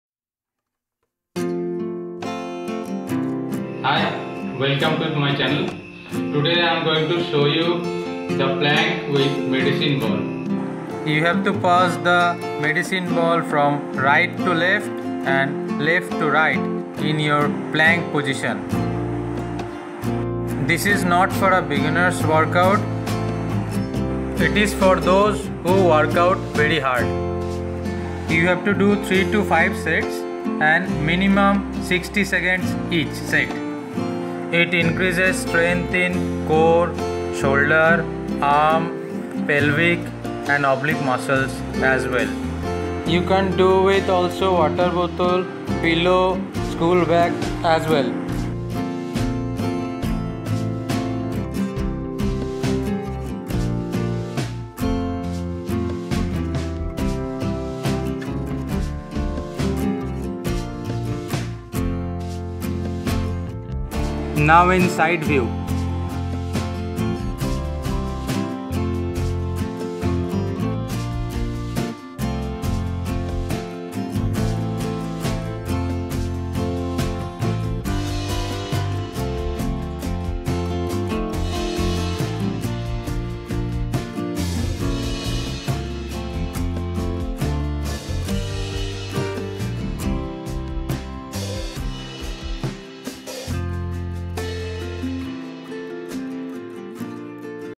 Med Ball Passing in Plank | Home Workout
Med Ball Passing in Plank | You can do with One or Two Litre Water Bottle, Pillow, 4-5 Books in Bag.